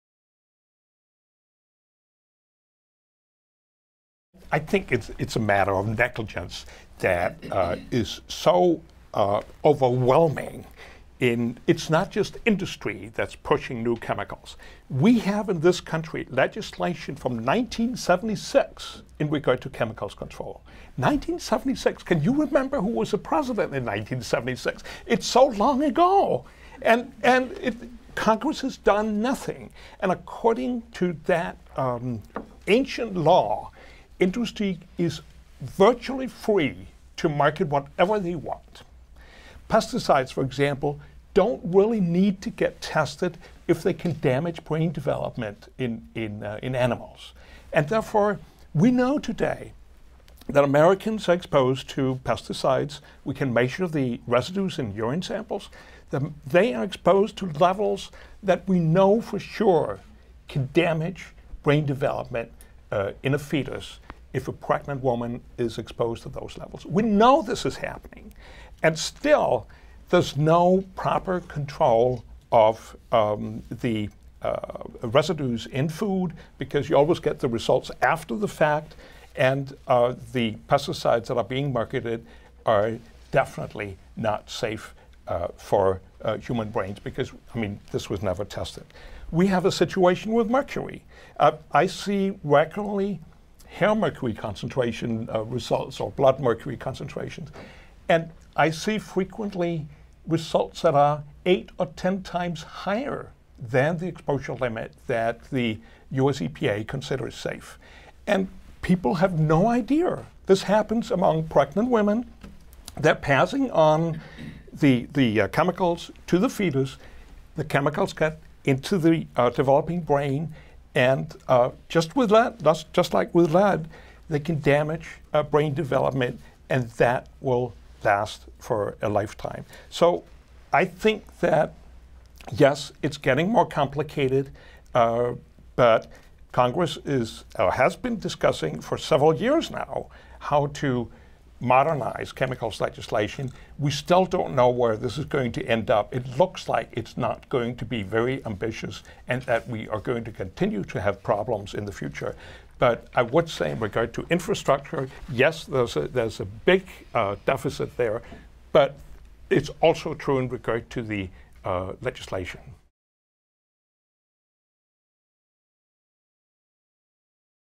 Chemical Regulation: Highlight from Chemicals and the Brain
Philippe Grandjean, Professor of Environmental Health at Harvard T.H. Chan School of Public Health, talks about the lack of government chemical regulation and oversight.

The water crisis gripping Flint, Michigan has exposed thousands of children to unsafe lead levels, triggering a federal emergency declaration and national conversation about basic public health protections. Lead can be toxic to the brain, and children can be particularly vulnerable. However, the Flint example is not unique; other American cities, including the nation’s capitol, have faced lead contamination in water supplies. And research has pointed more generally to an expanding list of chemicals, including certain pesticides, mercury and flame retardants, that may be linked to cognitive delays and health conditions in children. This Forum examined those links and the implications for both children and adults, while exploring public policy successes and failures in safeguarding the public’s health against neurotoxicants.

Part of The Andelot Series on Current Science Controversies, this Forum event was presented February 19, 2016 in Collaboration with PRI’s The World & WGBH.

Watch the entire series from The Forum at Harvard T.H. Chan School of Public Health